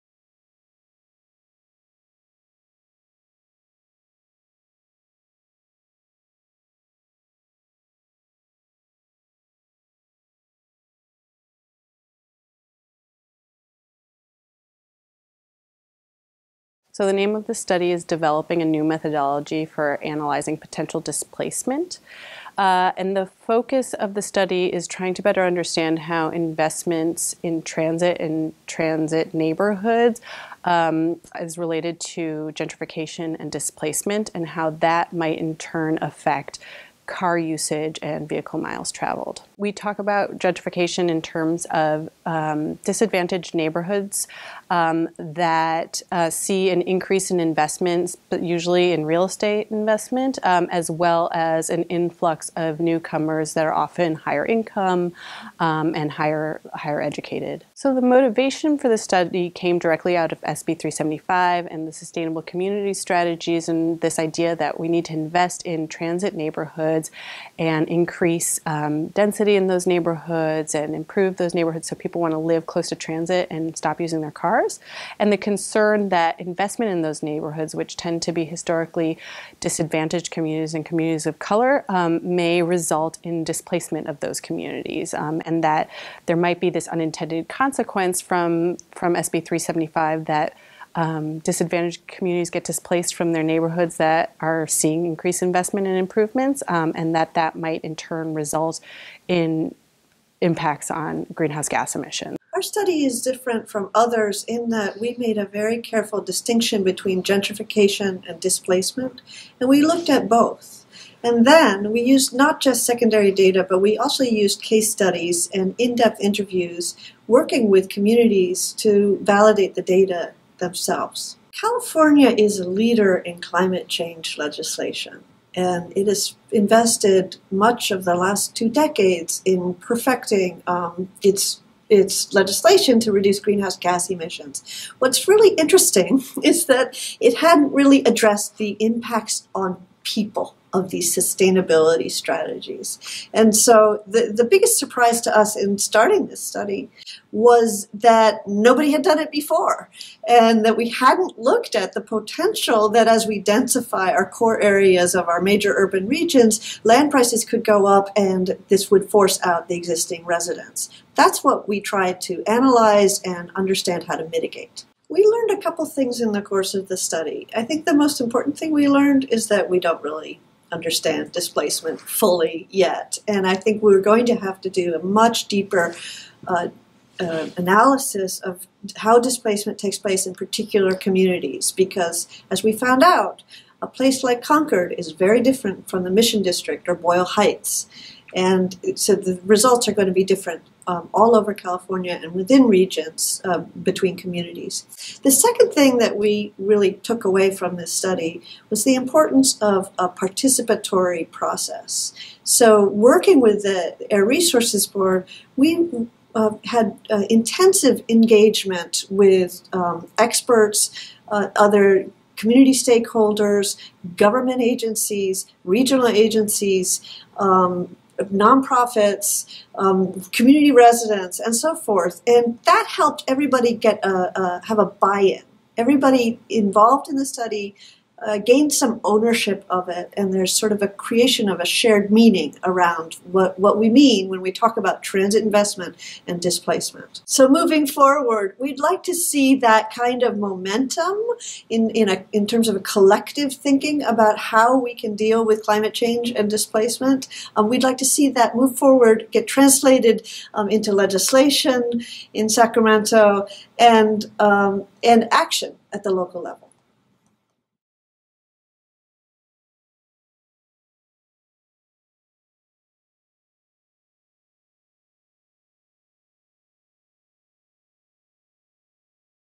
Research Seminar - Developing a New Methodology for Analyzing Potential Displacement
The study examined the relationship between fixed-rail transit neighborhoods and displacement in Los Angeles and the San Francisco Bay Area. The researchers modeled patterns of neighborhood change in relation to transit-oriented development (TOD), and found that TOD is associated with changes in the stability of the surrounding neighborhood, such as increases in housing costs and the loss of low-income households. The research found mixed evidence as to whether gentrification and displacement in rail station areas would cause an increase in auto usage and vehicle miles traveled (VMT).